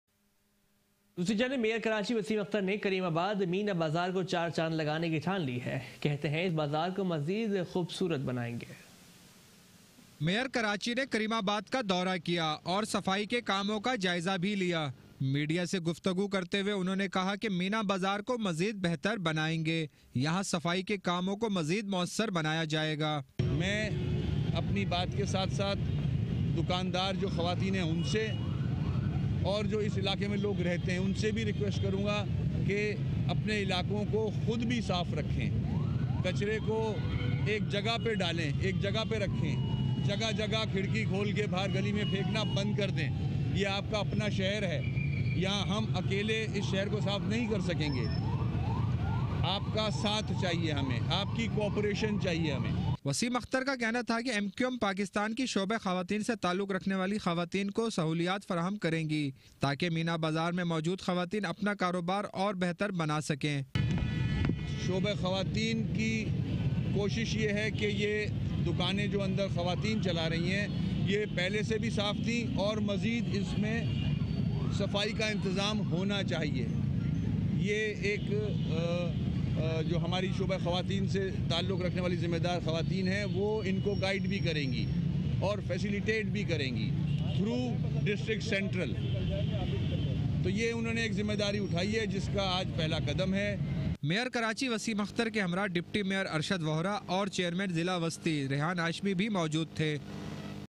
Mayor, Dy Mayor, Chairman Central visit Karimabad
Wasim Akhtar a visit to Karimabad on ... Mayor along with the DMC Central Chairman visited different roads of Karimabad